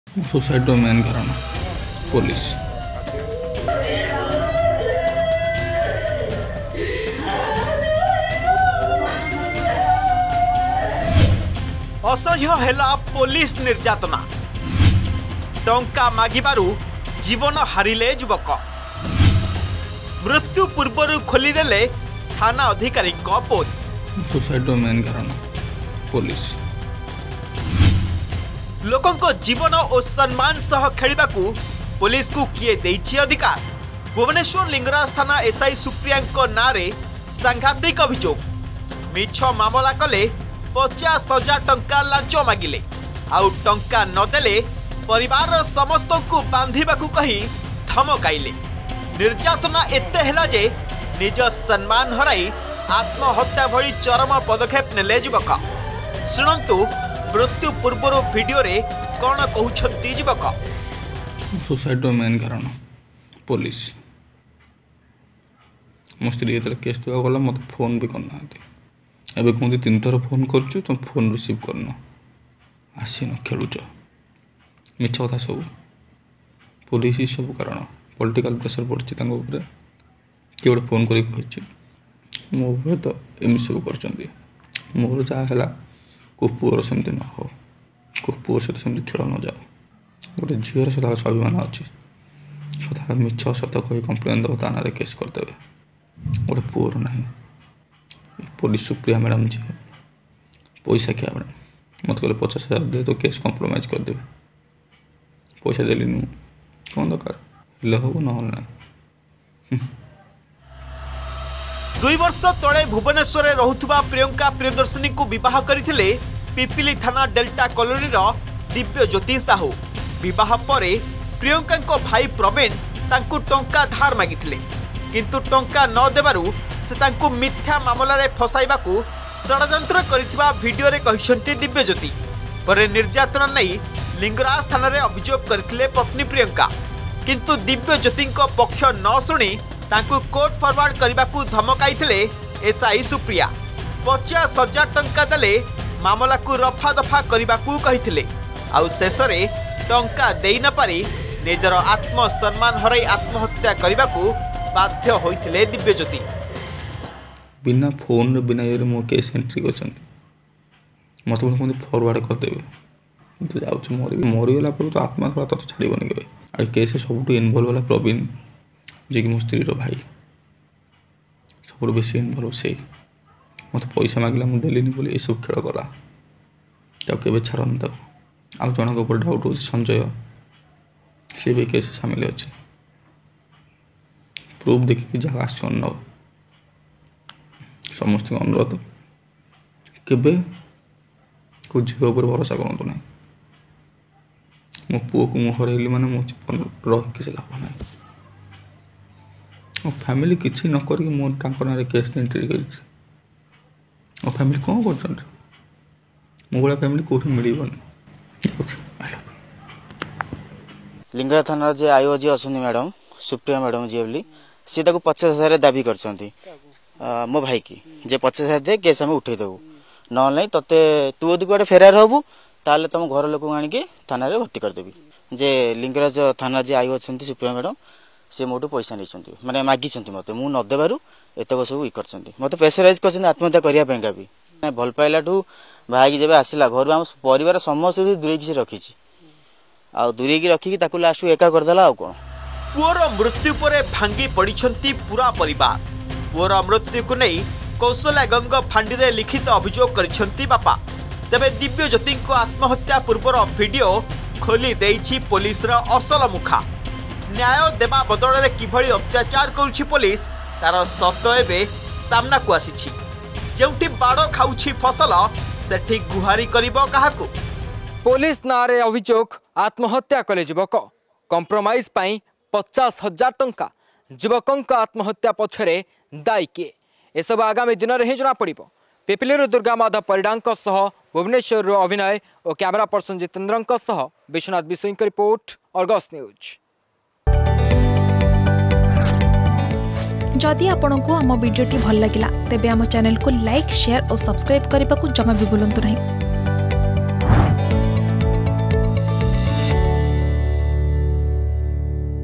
ଅସହ୍ୟ ହେଲା ପୋଲିସ ନିର୍ଯାତନା || Police Officers || Money
ଜୀବନ ହାରିଲେ ଯୁବକ । ମିଛ ମାମଲାରେ ପୋଲିସ ଅଧିକାରୀ ୫୦ ହଜାର ଟଙ୍କା ଲାଞ୍ଚ ମାଗିଥିବା ଅଭିଯୋଗ । ସାମ୍ନାକୁ ଆସିଲା ମୃତ୍ୟୁ ପୂର୍ବର ଭିଡିଓ । ଲୋକଙ୍କ ଜୀବନ ଓ ସମ୍ମାନ ସହ ଖେଳିବାକୁ ପୋଲିସକୁ କିଏ ଦେଲା ଅଧିକାର ?